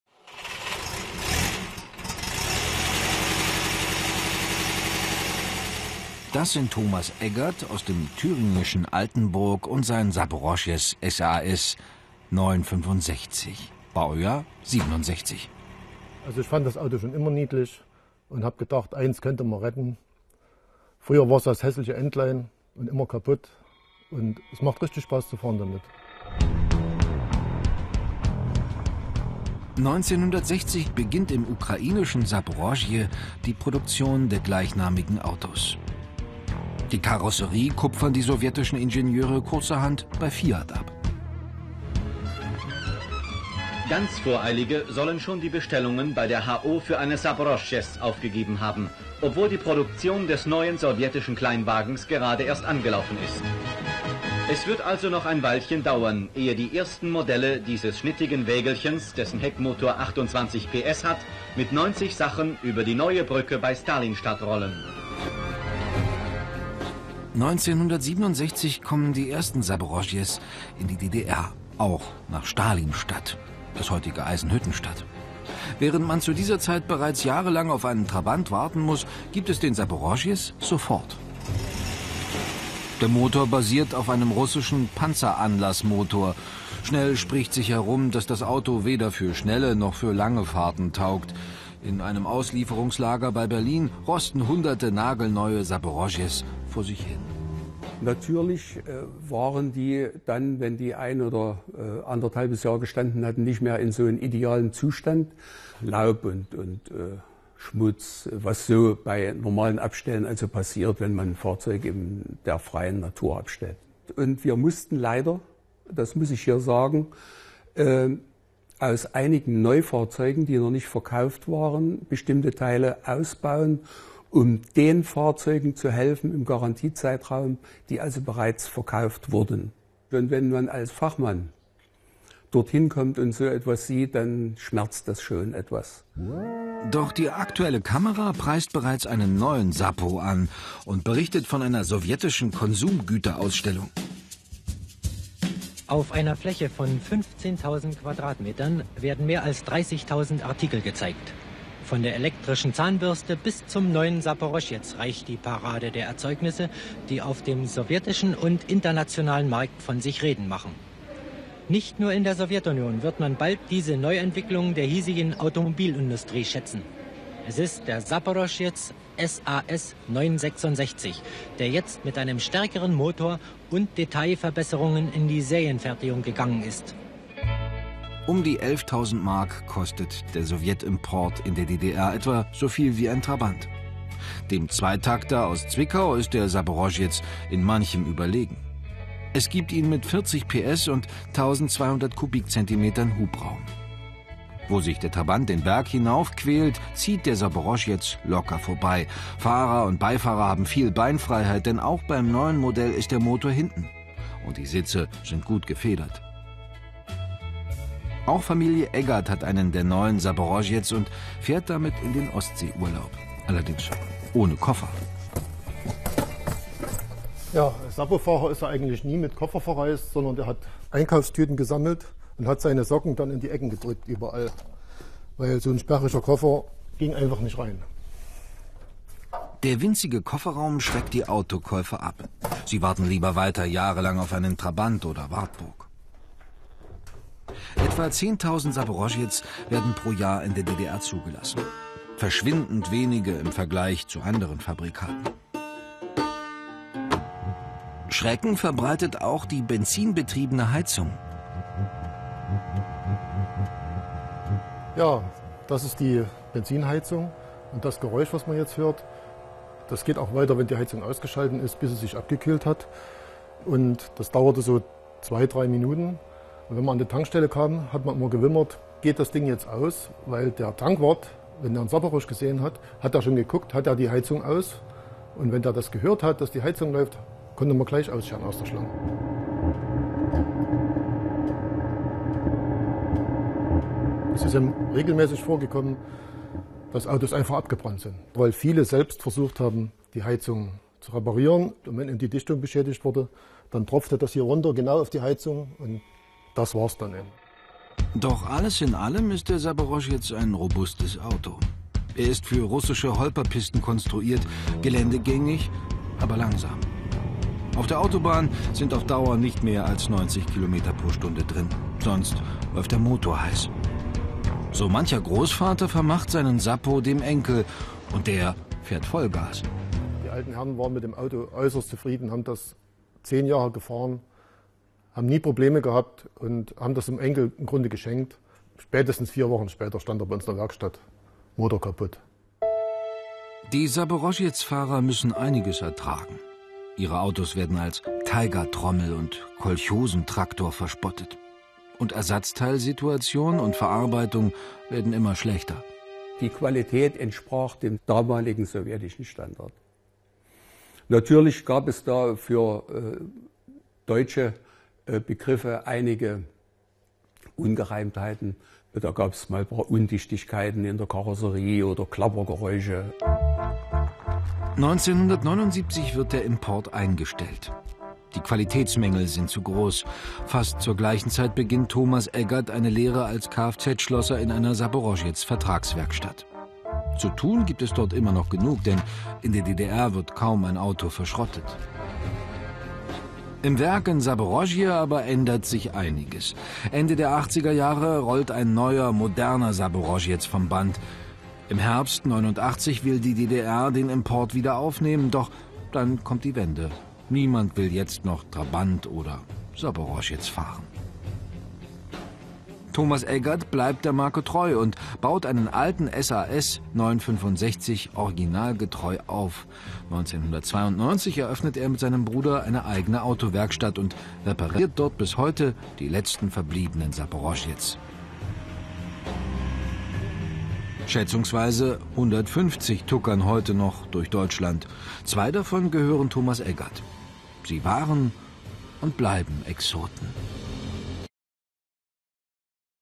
Fahrzeugen des Ostens, der Saporoshez
Der Saporoshez zählt zu den ungewöhnlichsten Vehikeln, die der sozialistische Fahrzeugbau hervorgebracht hat. Die Herzen der DDR-Autoliebhaber konnte er trotz kurzer Wartezeiten nicht gewinnen. Noch vor dem Ende der DDR wurde der Import des robusten Kleinwagens eingestellt. Eine kleine Fangemeinde gibt es allerdings immer noch.

Die DDR-Bürger hatten die Autoimporte aus den sozialistischen Bruderländern zu schätzen gelernt. Denn es gab kaum Alternativen, um der geringen Modellauswahl und den langen Wartezeiten aus dem Weg zu gehen. Eines dieser Importautos war der Saporoshez. Doch an keinem anderen Wagen schieden sich die Geister so sehr wie am "Sapo".

Die Geschichte des Sapo begann Ende der 1950er Jahre in dem Kommunarwerken der ukrainischen Stadt Saporischschja. Wo seit Mitte des 19. Jahrhunderts Landwirtschaftsmaschinen hergestellt worden waren, sollte fortan ein robuster Kleinwagen vom Band rollen. Längst hatten sich die sowjetischen Automobilbauer den Ruf erworben, sich einiges vom westlichen Fahrzeugbau abzuschauen. Ihre wahre Kunst war es aber, Technik und Design so anzupassen, das die Autos den schlechten Witterungen und Straßenverhältnisse trotzen konnten.

Export

Der Saporoshez wurde seit den sechziger Jahren in Belgien unter der Bezeichnung Yalta, in den Niederlanden, Dänemark und Finnland – Jalta, in Österreich – Eliette und in einigen westeuropäischen Ländern (u. a. Belgien, Niederlande, Italien) später als ZAZ angeboten.
Für die meisten Exportmärkte wurde jedoch der Originalname beibehalten und dessen Schreibweise an die jeweilige Landessprache angepasst.
Wie der Moskwitsch wurde der SAS 966 zeitweise auch in Belgien als Yalta 1000 montiert, ein Teil der dort hergestellten Wagen wurde mit dem Motor des Renault 8 ausgestattet.